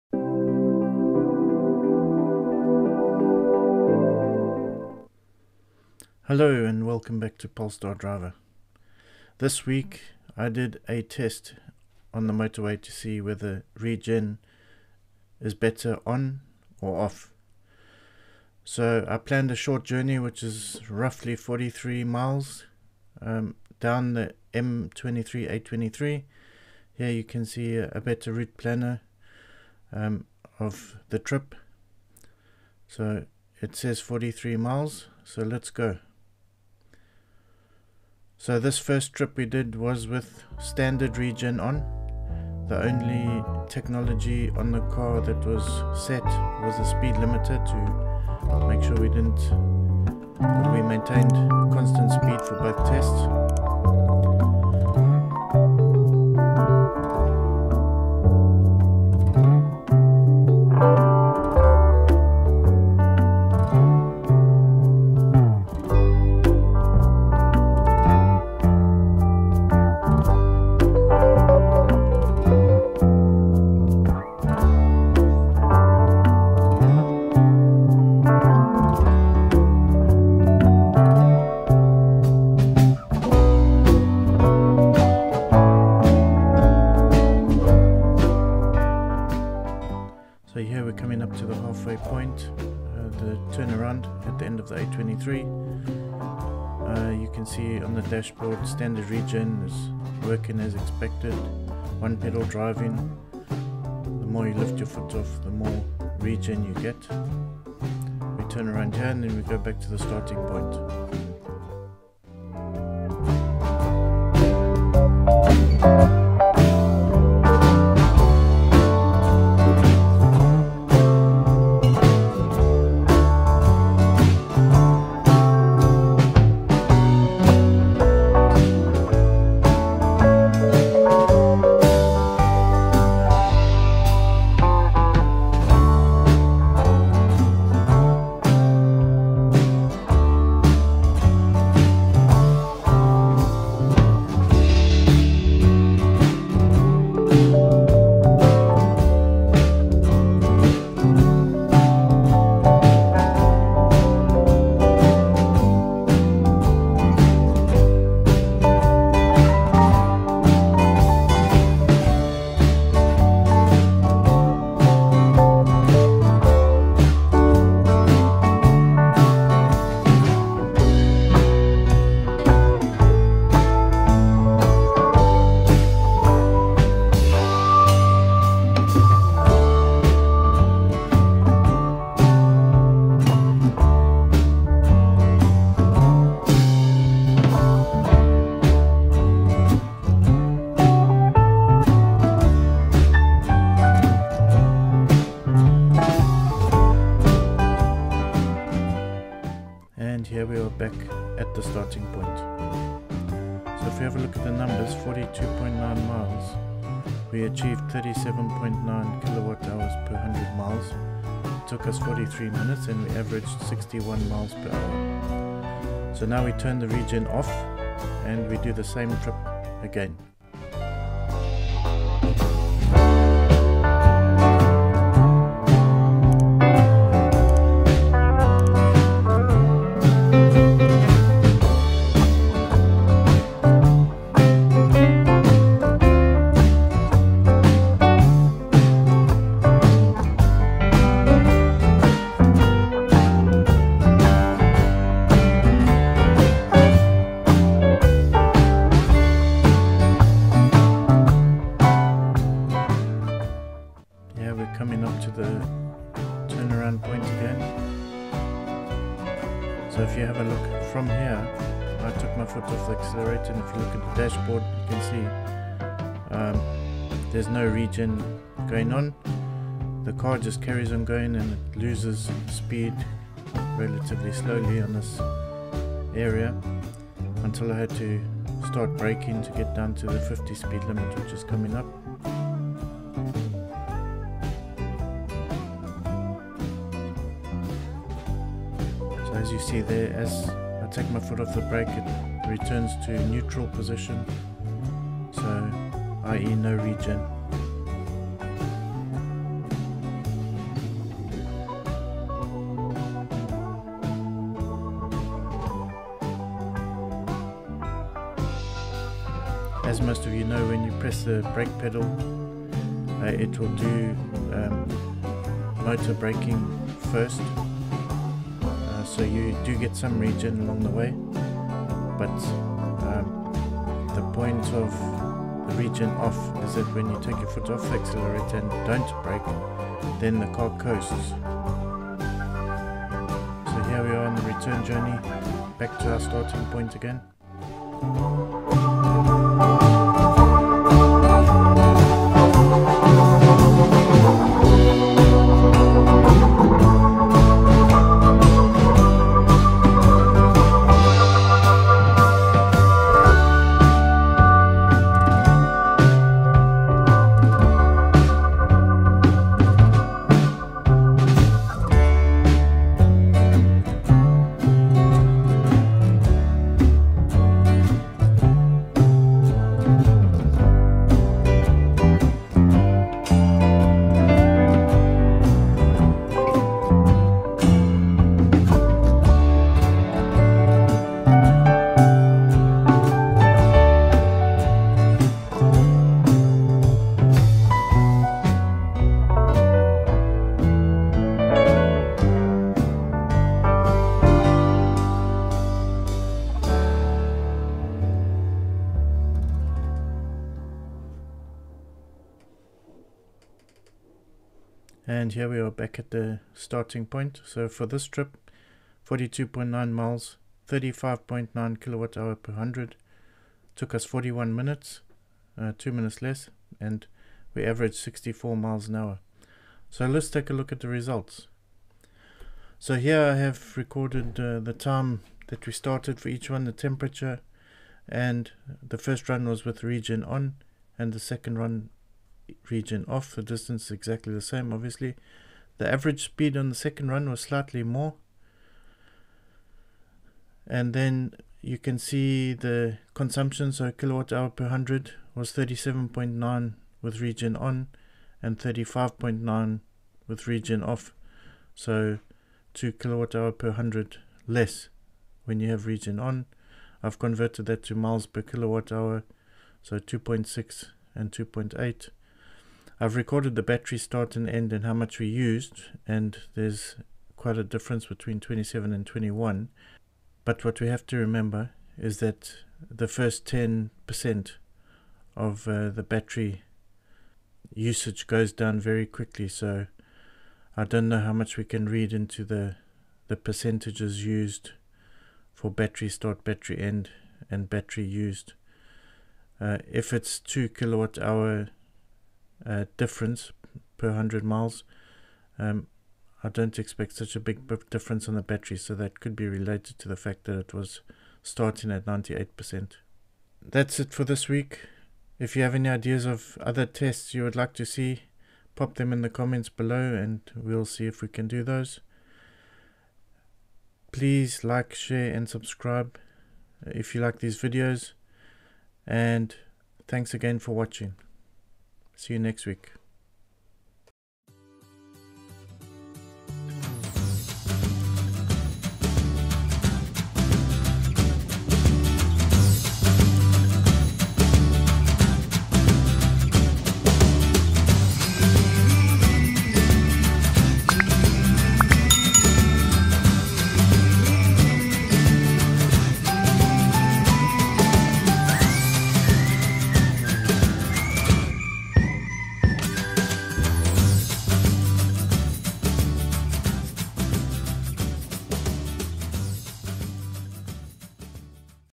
Polestar 2 - Is motorway consumption better with OPD on or off?
This week I did a back-2-back test on the same trip with regen on and off to test which is more efficient for motorway driving.  Take a look at the tests and results at the end of the video.  Let me know your thoughts and comments on this topic.